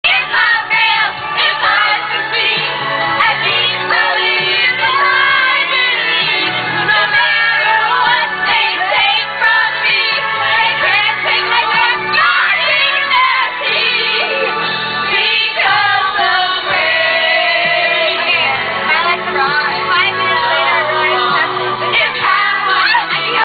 watch out whitney
the girls belt one out after some champagne and the limo ride of a lifetime...